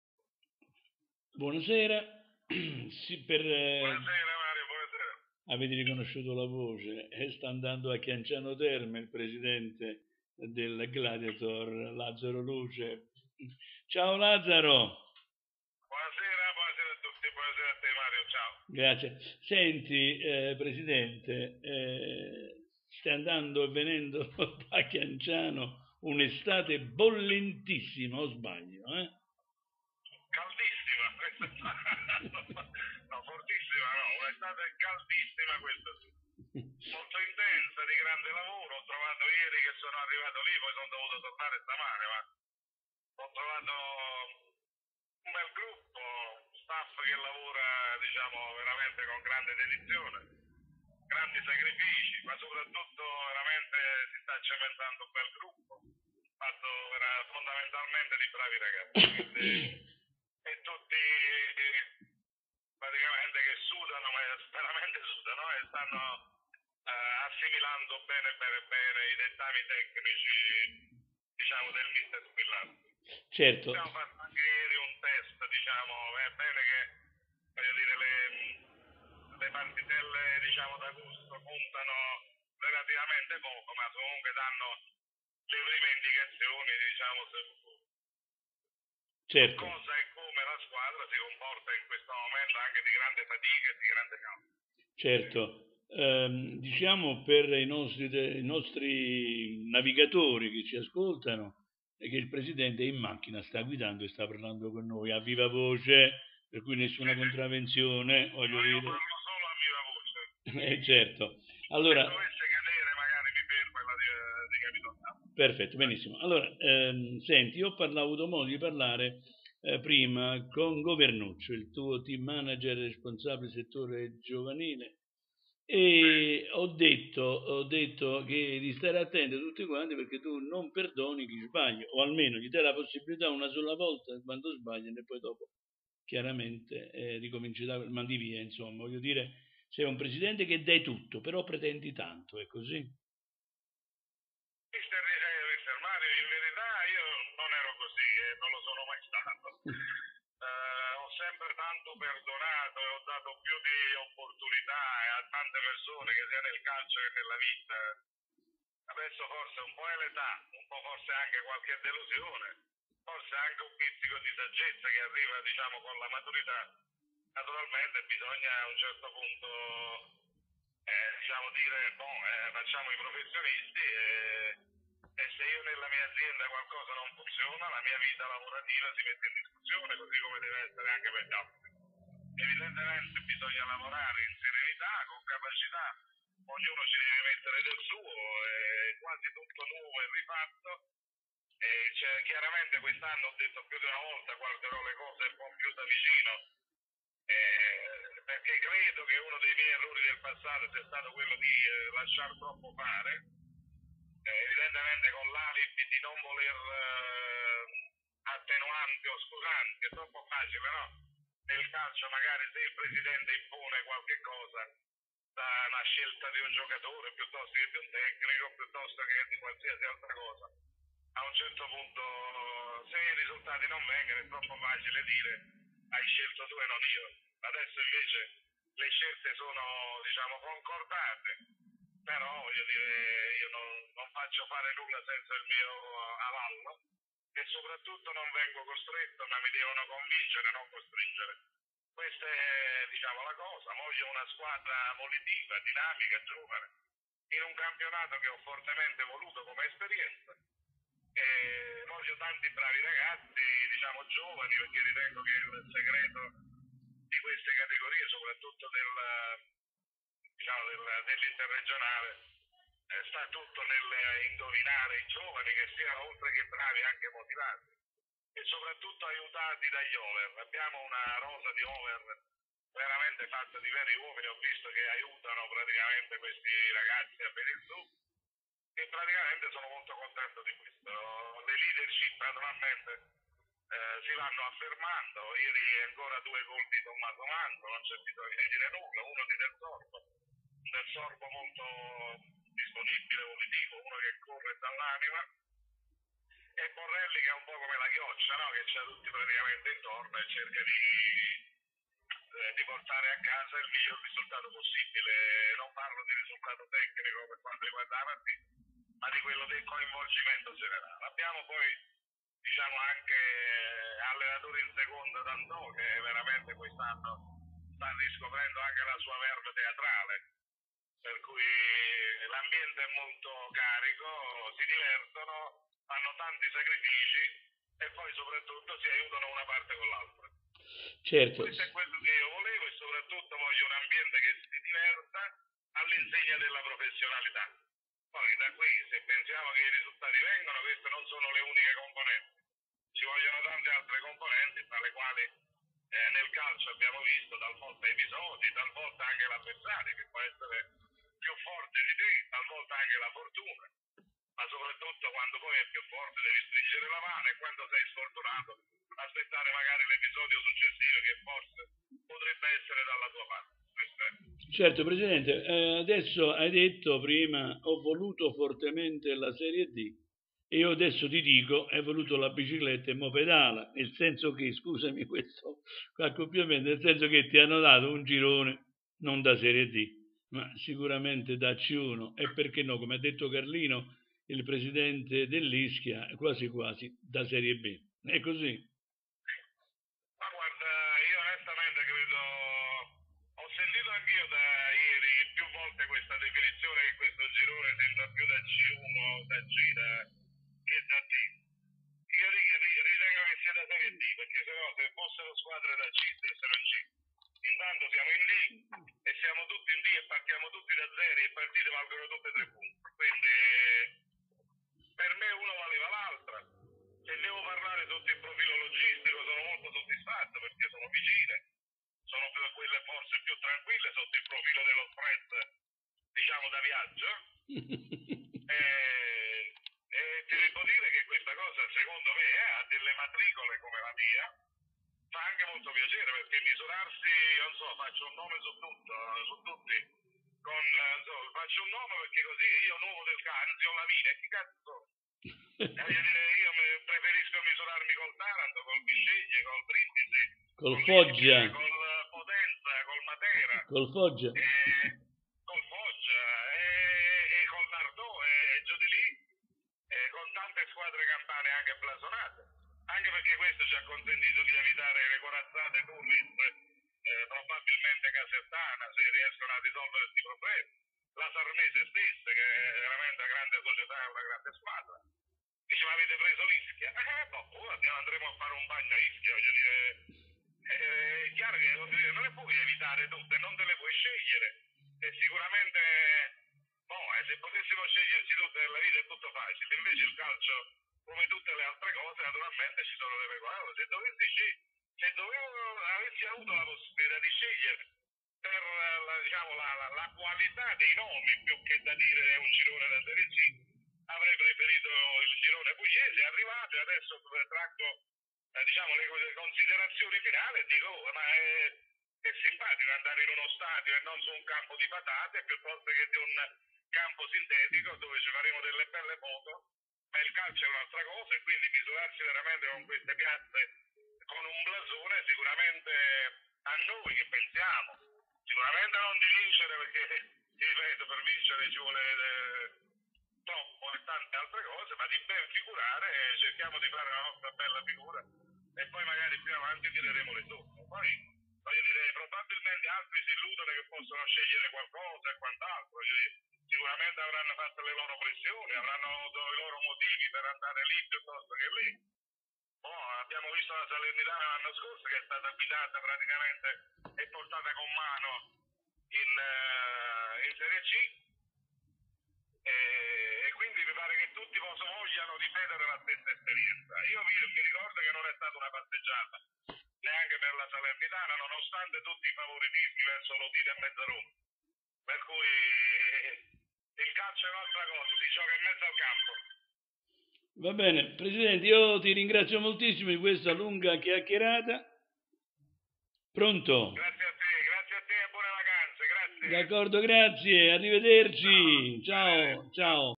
GLADIATOR - AL VIDEOTEL IL PRESIDENTE LUCE per Golden Gol una Serie D da Serie A
Lazzaro Luce "Confrontrarci con grandi piazze qualifica il Gladiator e la categoria che ho fortemente voluto"